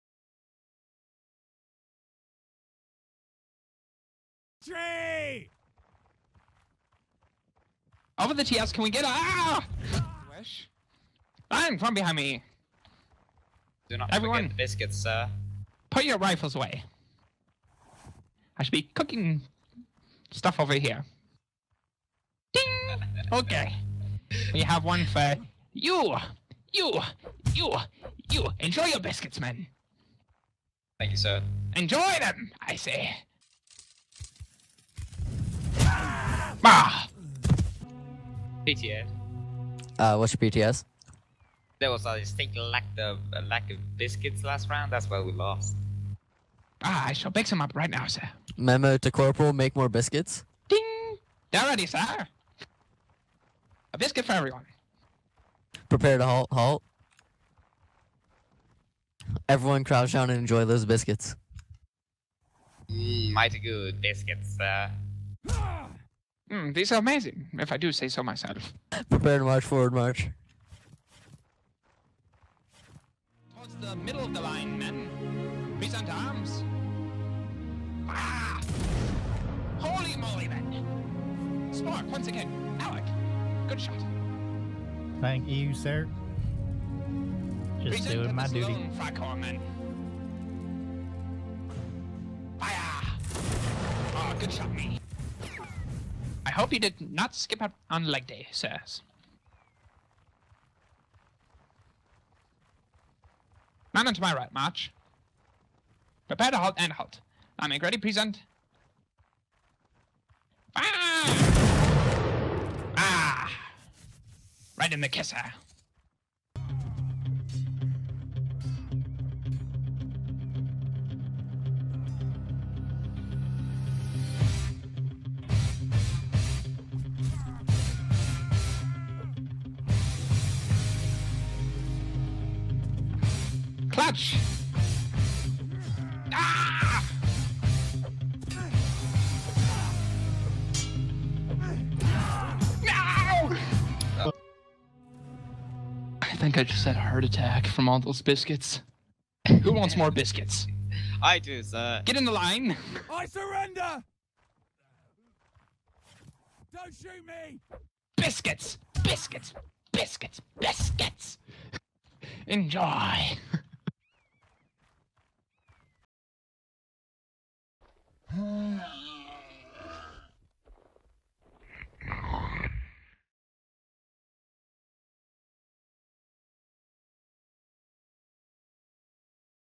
Skirmish Fun: Johann's Accents - 9th East Norfolk Regiment of Foot
Although we are a very serious and professional regiment, who doesn't like a good laugh?

video by alec